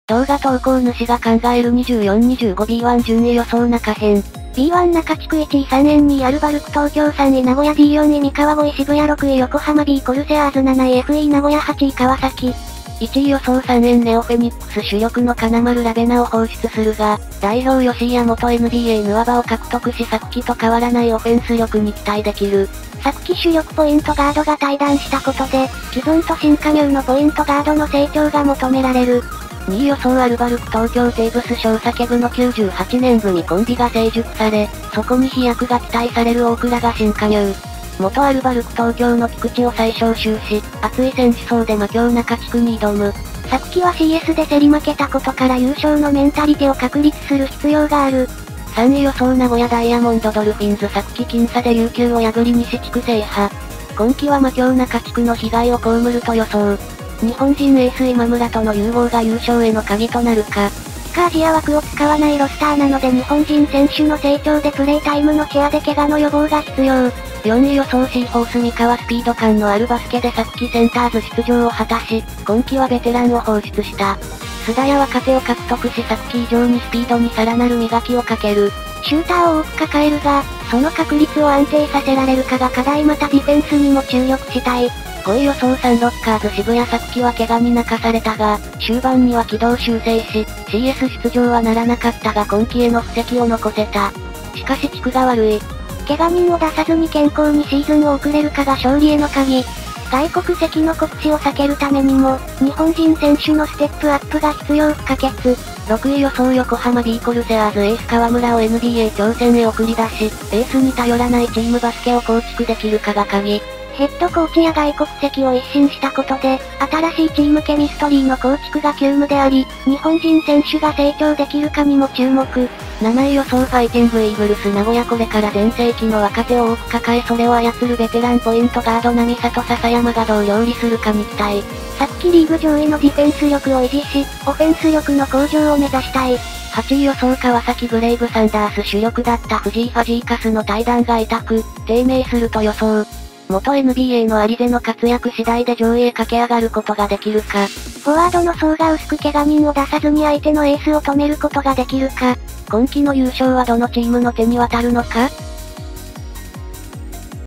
24-25B1順位予想 中地区編
久田 正雄
ロバーツ・ケイン
コー・フリッピン
ハンター・コート
モサク・ダミロラ
アイザイア・マーフィー
ザック・バランスキー
ミサカボパカ ベニ
ジャワラ ジョセフ
鎌田 裕也
ペリン・ビュフォード
ジョン・ムーニー
菅野 ブルース
クベマ・ジョセフ・スティーブ
酒井 達晶
小室 昂大
横浜ビー・コルセアーズ